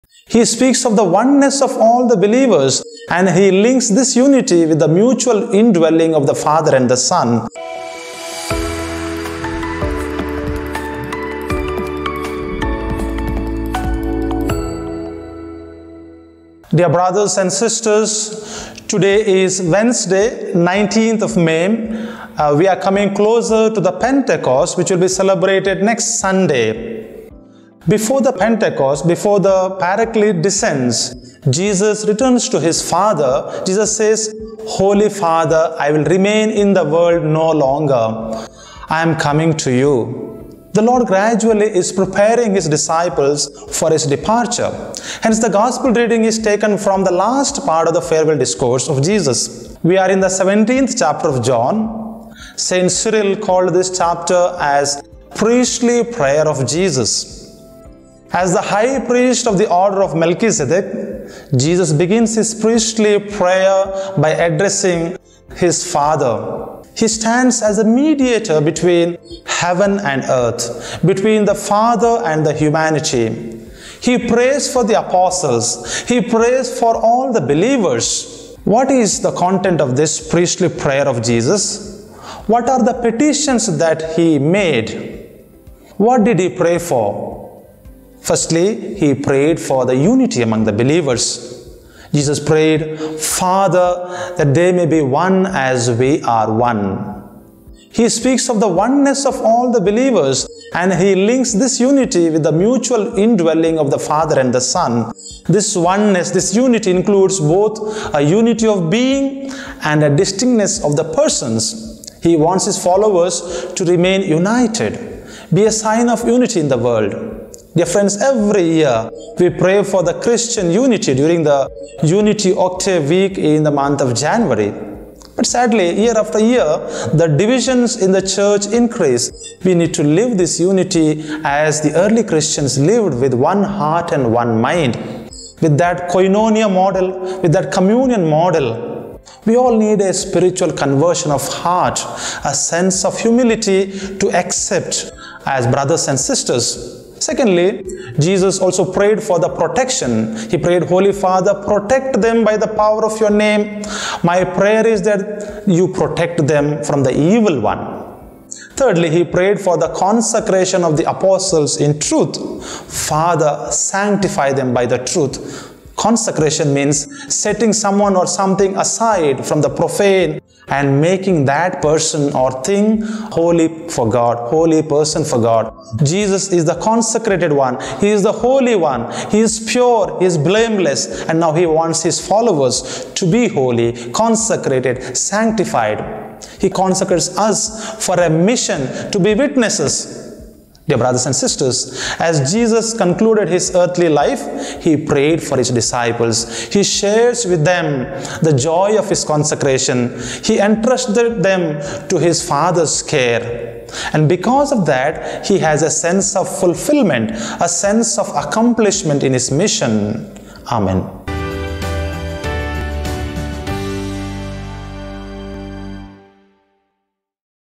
19/05/2021 Reflection 1175 (Fr Cyril Walder OP on John 17:11-19)
Dear brothers and sisters, today is Wednesday, 19th May. We are coming closer to the Pentecost which will be celebrated next Sunday. Before the Paraclete descends, Jesus returns to His Father. Jesus says, “Holy Father, I will remain in the world no longer. and I am coming to you.” The Lord is gradually, preparing his disciples for His departure. Hence the Gospel reading is taken from the last part of the Farewell Discourse of Jesus. We are in the 17th Chapter of John. St. Cyril Called this chapter as “Priestly Prayer of Jesus”.
As the High Priest of the order of Melchizedek, Jesus begins His Priestly Prayer by addressing his Father. He stands as a Mediator between heaven and earth, between the Father and the humankind. He prays for his apostles, he prays for all the believers.
What is the content of this Priestly prayer? What are the petitions he made? What did he pray for?
Firstly, he prayed for the Unity among the believers. He prayed, “Father, they may be one as we are one.” He speaks of the oneness of all the believers and links this unity with the mutual indwelling of the Father and the Son. This oneness includes both a Unity of being and distinctness of persons.
He wants his followers to remain united, and be a sign of unity in the world. Every year, we pray for the Unity during the Unity Octave Week in January, but sadly, year after year, the divisions in the Church increase. We need to live in unity as the early Christians lived with “one heart and one mind”, with that Koinonia or communion model. We all need a Spiritual conversion of heart and a sense of humility.
Secondly, he prayed for the Protection: “Holy Father, protect them by the power of your name… My prayer is that you protect them from the evil one”.
Thirdly, he prayed for the Consecration of the apostles in truth: “Father, Sanctify them by the truth.”  Consecration means setting someone or something aside from the profane and making that person or thing ‘holy for God’.
Jesus is the Consecrated one, He is Holy, Pure, blameless, and now He wants his followers to be holy, consecrated, and sanctified. He consecrates us for a mission...
Dear brothers and sisters, as Jesus concluded his earthly life, he prayed for his disciples, he shared with them the joy of his consecration. He entrusted them to His Father’s care.
Because of that, he has a sense of fulfillment, a sense of accomplishment.  Amen.